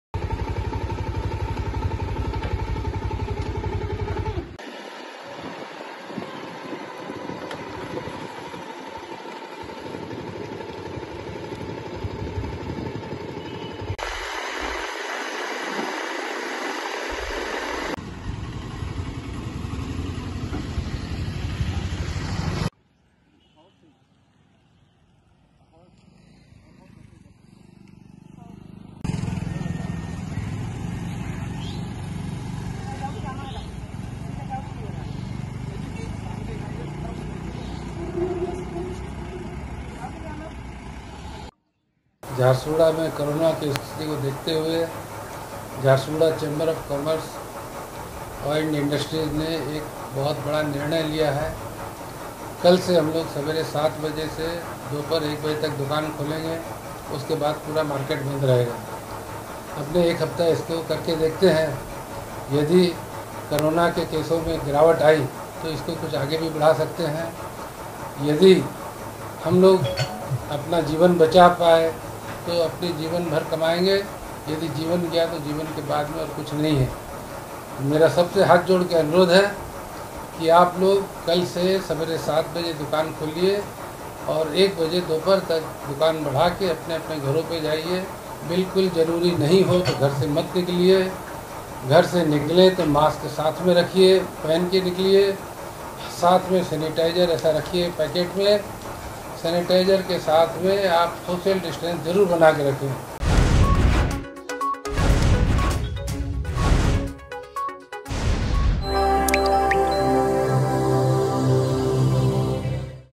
କରୋନା ସଂକ୍ରମଣ ଚେନକୁ ଭାଙ୍ଗିବା ଲାଗି କଟକଣାକୁ କଡାକଡି କରିଛନ୍ତି ସରକାର ...
ଝାରସୁଗୁଡା ଜିଲ୍ଲାର ସମସ୍ତ ସହରାଞ୍ଚଲରେ ରାତ୍ରୀକାଳୀନ କର୍ଫ୍ୟୁ ସଂଧ୍ୟା 6ଟାରୁ ଆରମ୍ଭ ହୋଇଥିବା ବେଳେ ସପ୍ତାହନ୍ତ ସଟଡାଉନ୍ ଜାରୀ ରହିଛି...